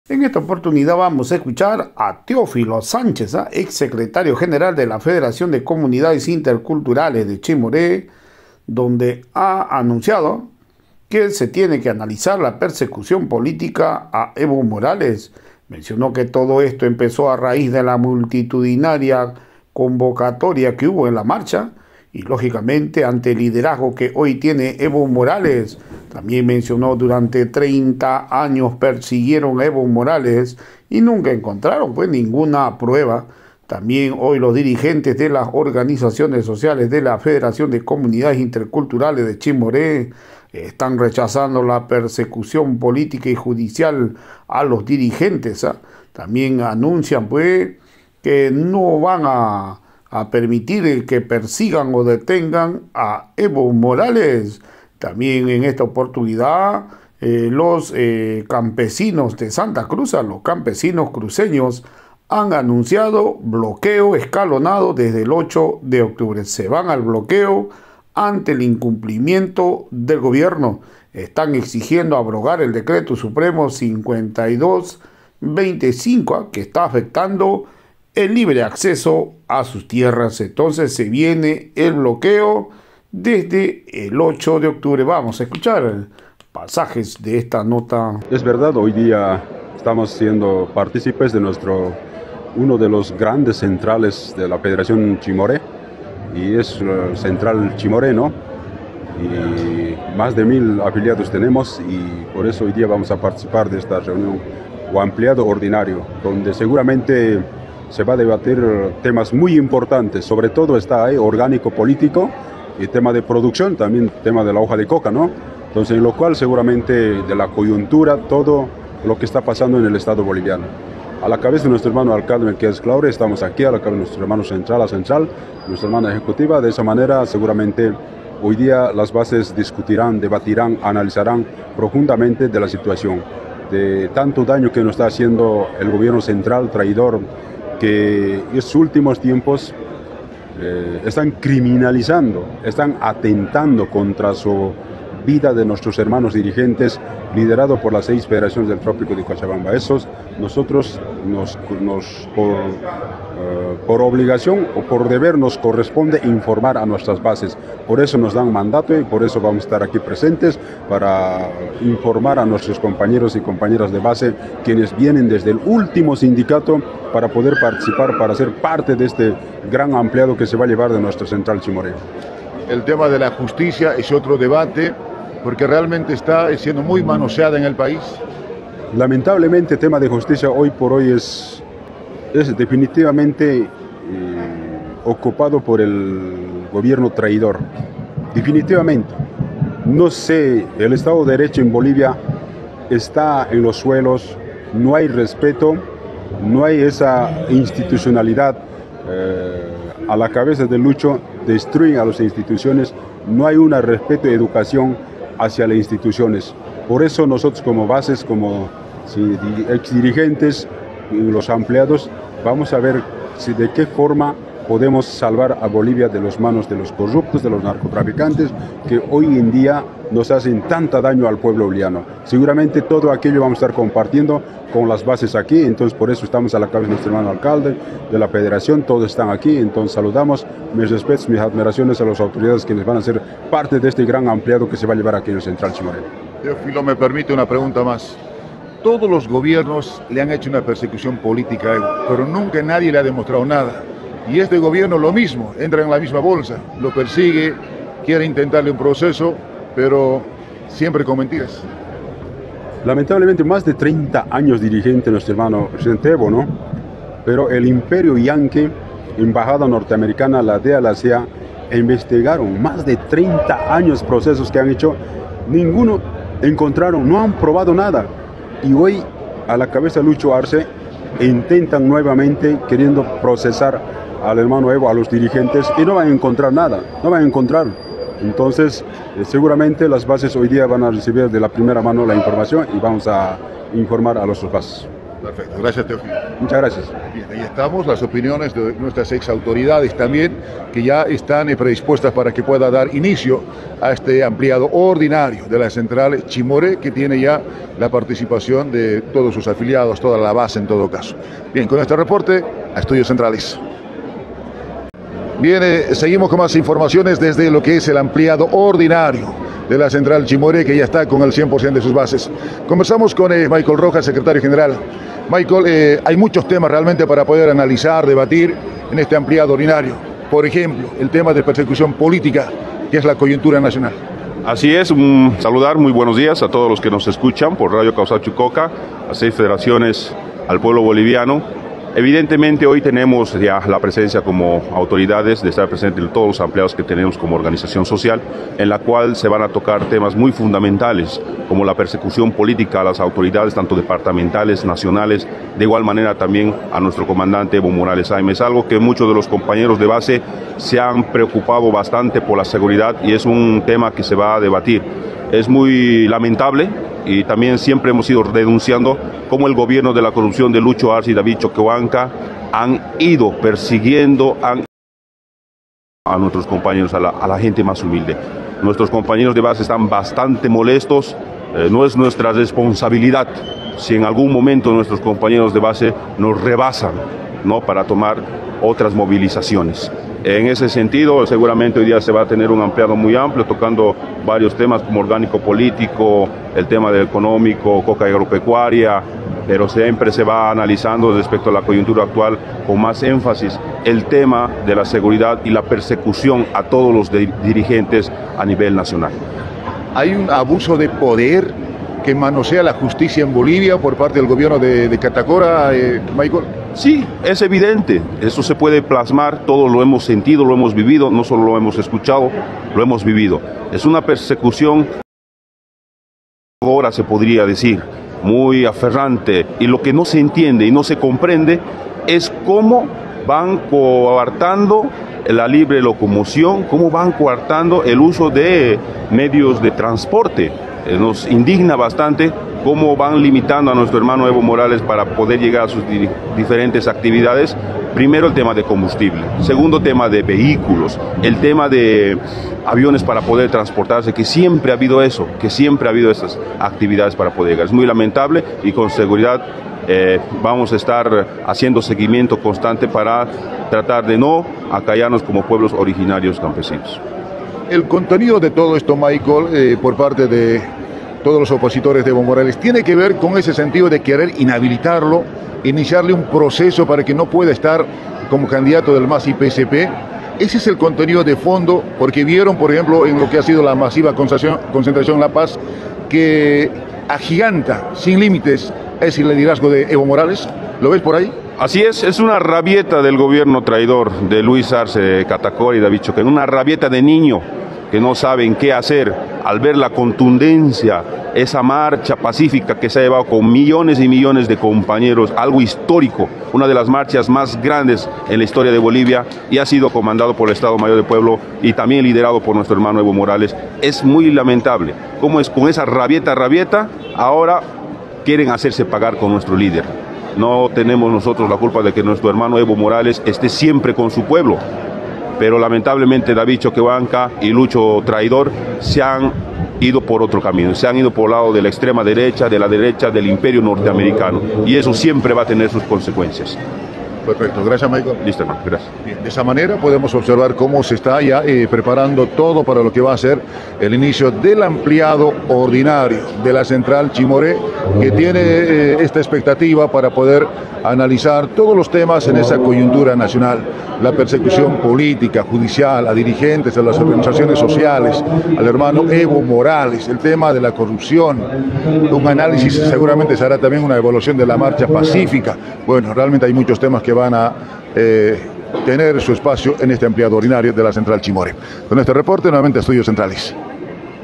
EVO MORALES FUE PERS3GUIDO POR GOBIERNO POR LA MULTITUDINARIA CONVOCATORIA PARA LAS MARCHA..
CUESTIONAN LUCHO Y DAVID POR HABERSE IDO CON LA DER3CHA..